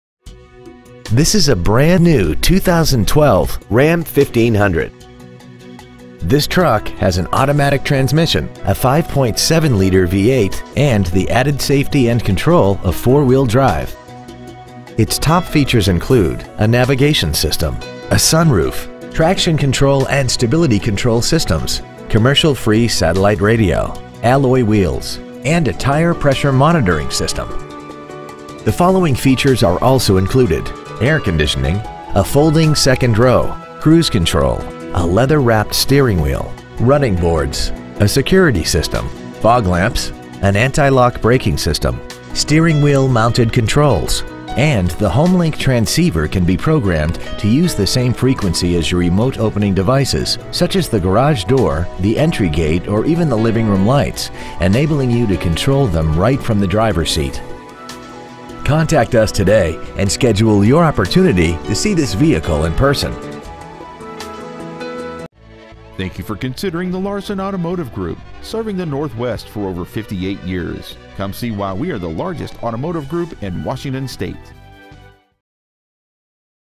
2012 Ram 1500 Puyallup WA
Year : 2012
 Make : Ram
 Model : 1500
 Engine : 5.7L V8 OHV 16V
 Trans . : Automatic
 Exterior : Bright Silver Metallic
 Miles : 25
 Interior : Dark Slate Cloth
 Stock : 305691

 2012 Ram 1500 Puyallup WA